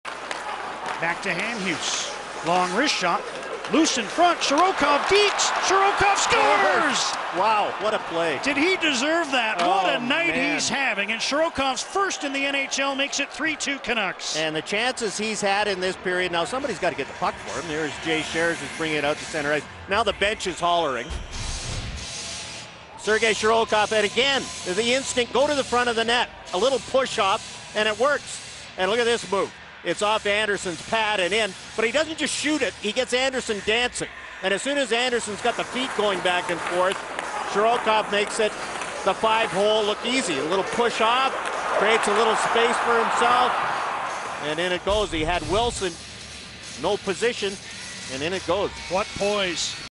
Sergei Shirokov First NHL Goal - Canucks at Avs - 01.18.11 - HD
Sergei Shirokov's first career NHL goal is a beauty as he gives the Canucks a 3-2 lead.

This video is property of the NHL & Rogers Sportsnet.